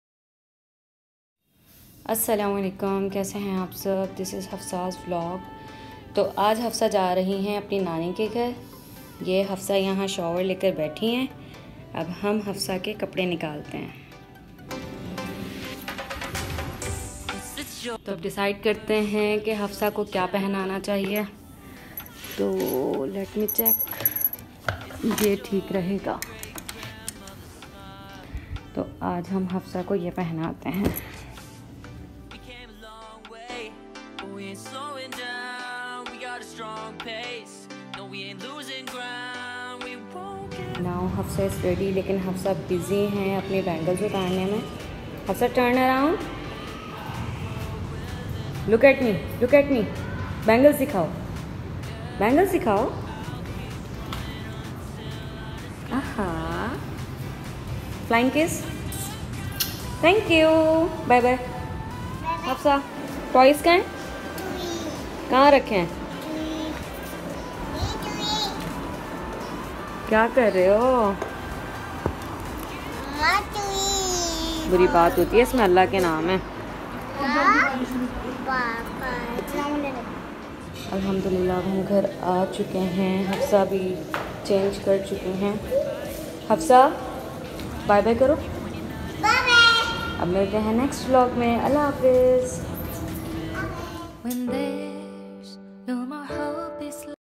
Nani’s Day | Hafsa Abid Vlogs
As.salam-o-Alaikum

And its hafsa’s second vlog in which she is getting ready for going to her nani 😋 Some short clips i’m am sharing with u guys ‘ If u like then don’t forget to like & subscribe 👍

Keep loving